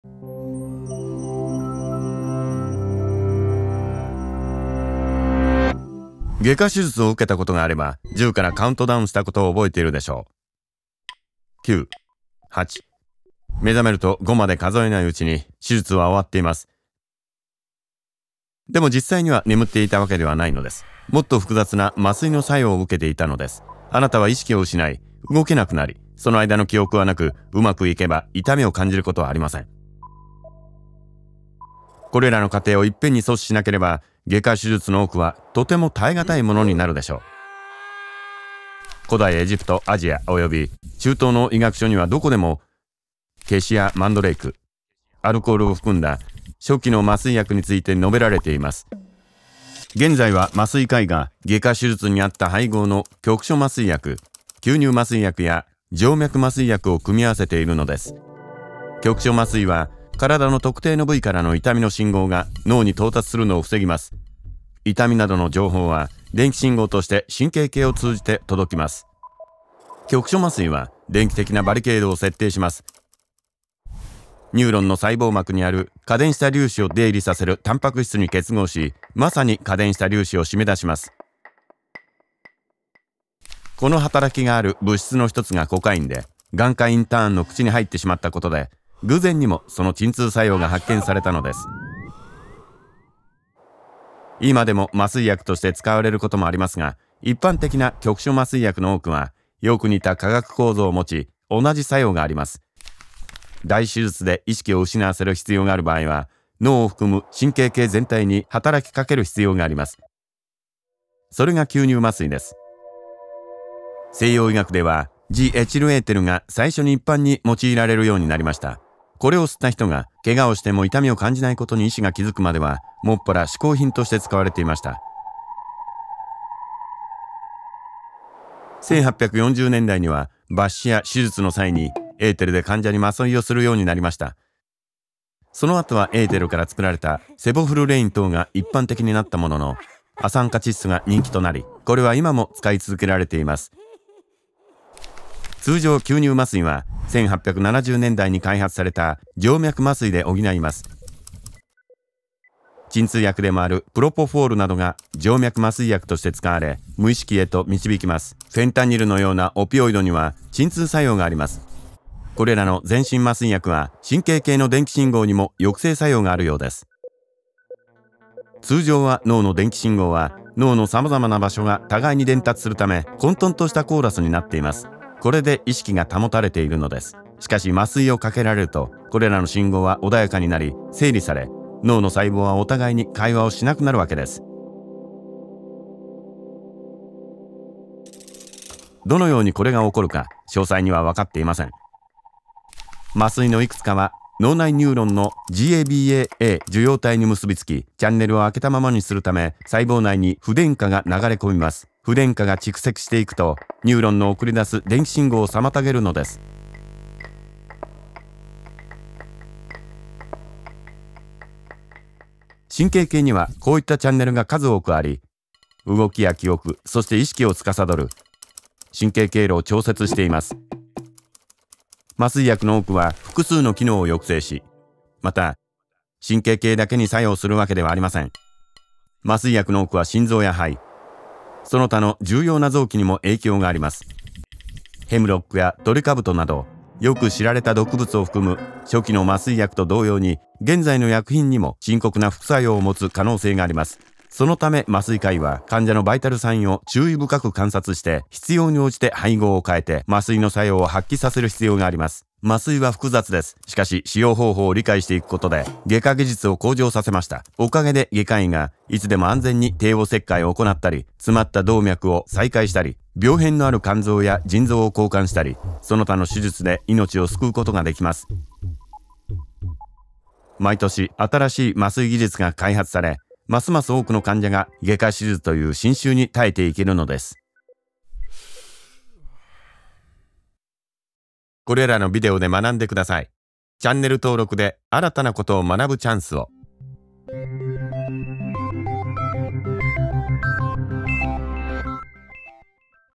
麻酔の仕組みとは？ ― スティーブン・チェン
麻酔を受けると、動けなくなり、記憶も形成されず、うまくいけば痛みも感じません。その間に眠っているように見えるかもしれませんが、実際にはそうではありません。では、何が起こっているのでしょうか？スティーブン・チェンが麻酔の科学について解説します。

Steven Zheng によるレッスン、Zedem Media 監督。